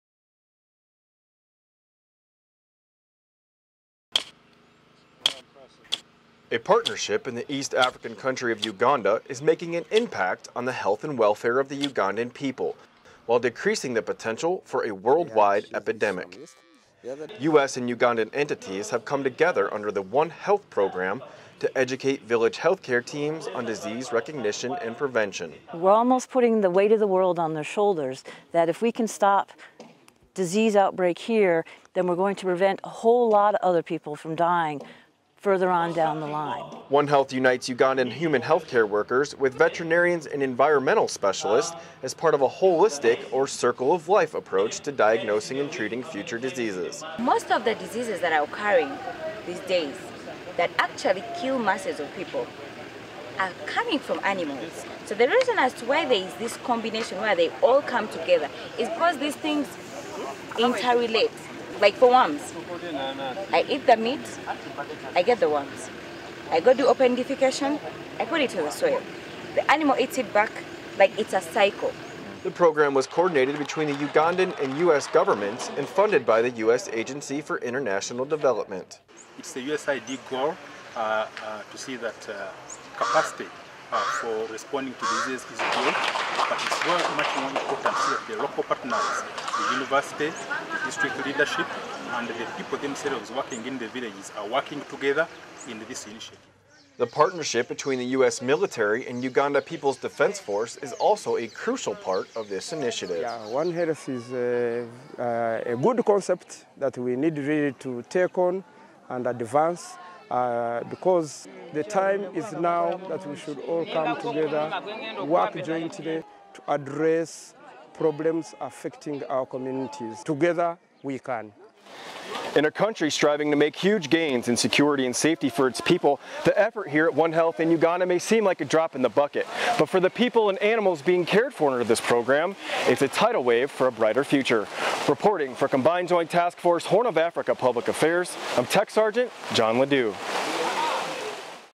U.S., Ugandan One Health Program Educates Health Care Workers | MiliSource
U.S. service members are doing their part to keep the world healthy. Tech. Sgt. Jon LaDue takes us to Uganda, Africa where Soldiers are working with local health care teams to prevent future outbreaks of disease. Includes sound bites from Maj. Thamus Morgan, U.S. Army Veterinarian, Mirembe Bernadette Basuta, Environmental Health Officer, Humphrey Kabugo, USAID Project Officer and Maj. Dr. Godwin Bagyenzi, Uganda People's Defense Force. Video by Tech. Sgt. Jon LaDue | Combined Joint Task Force - Horn of Africa | Date: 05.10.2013